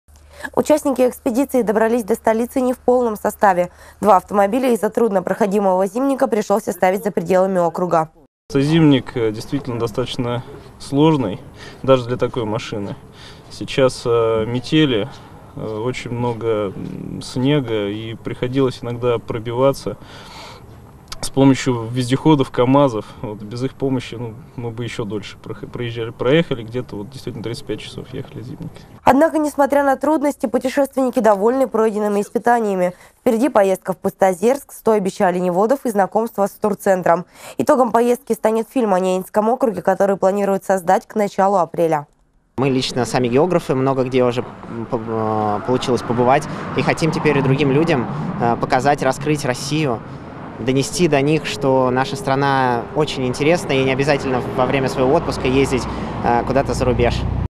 Экспедиция «Бросок в Заполярье» добралась до Нарьян-Мара не в полном составе
Несколько машин экспедиции «Бросок в Заполярье» не смогли преодолеть зимник Нарьян-Мар — Усинск. Из-за сильных метелей дорога оказалась проходима только для полноприводных грузовиков. Как пояснили в ГУ МЧС по НАО, в обе стороны идет одна колея, высота которой не позволяет двигаться по дороге легковым автомобилям и внедорожникам.